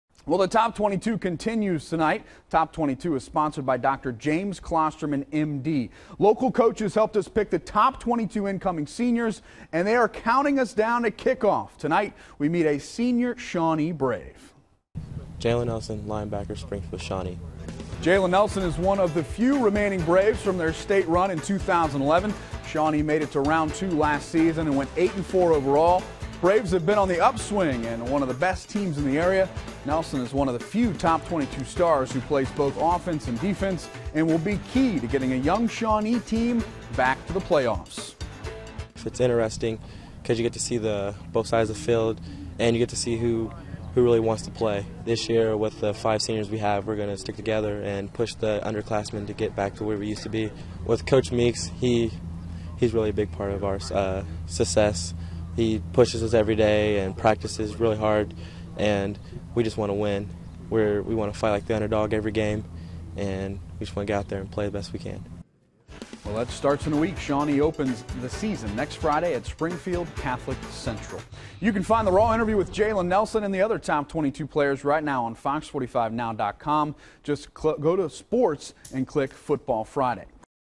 TOP 22 - Shawnee's Jalen Nelson working both side to make another run at state
SPRINGFIELD -- Jalen Nelson is one of the few remaining Braves from their state run in 2011.    Shawnee made it to round two last season and went 8-4 overall.    The Braves have been on the upswing and one of the best teams in the area..    Nelson is one of the few Top 22 stars who plays both offense and defense and will be key to getting a young Shawnee team back to the playoffs.Shawnee opens the season next Friday at Springfield Catholic Central.